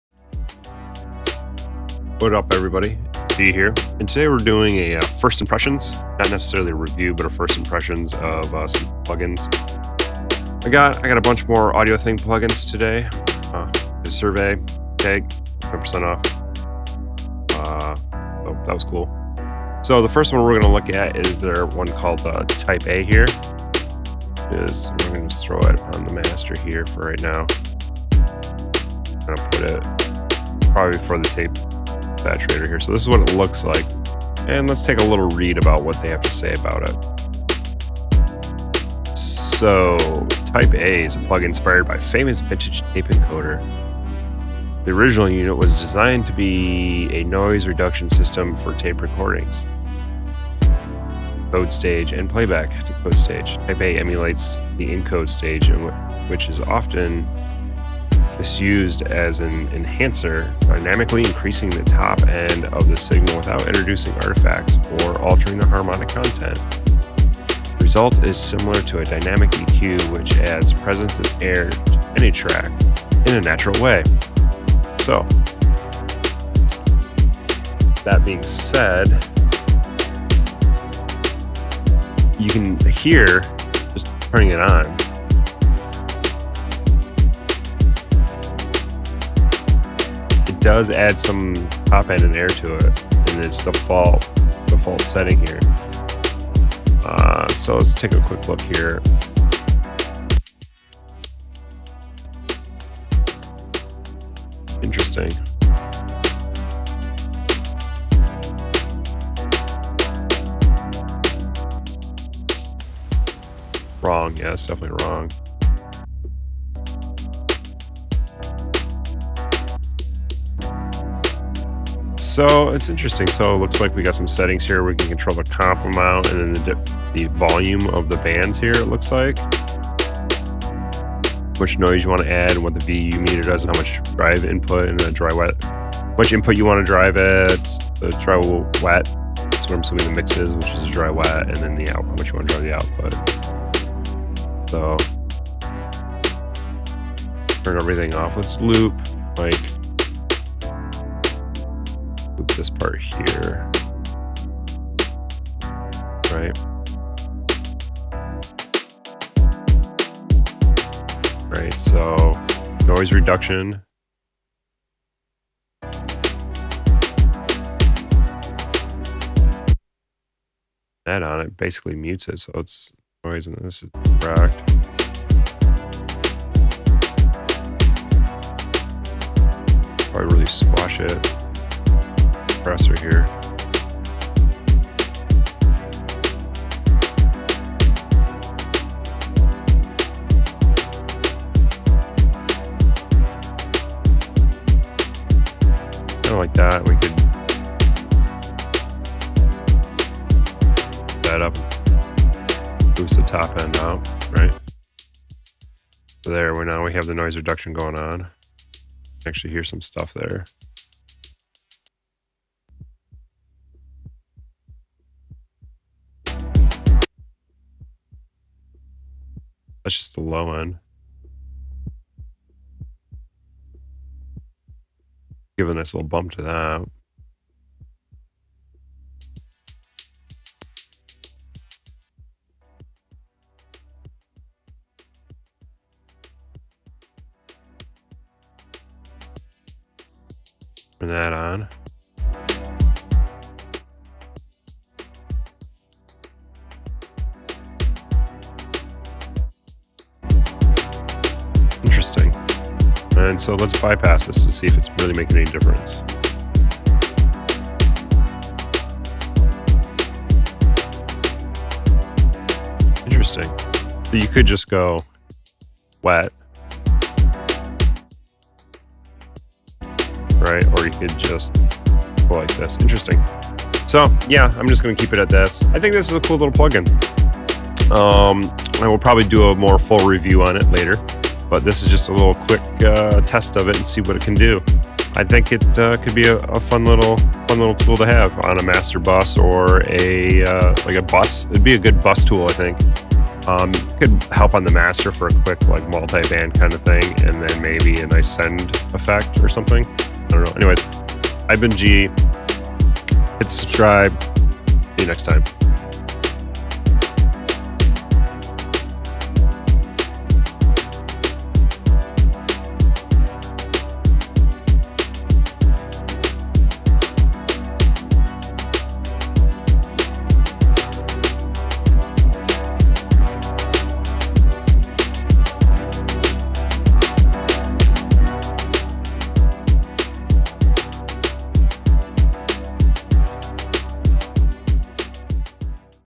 AudioThing TYPE A Quick Look.
Type A is a plugin inspired by a famous vintage tape encoder. The original unit was designed to be a noise reduction system for tape recording (encode stage) and playback (decode stage).

SUPPORT ME!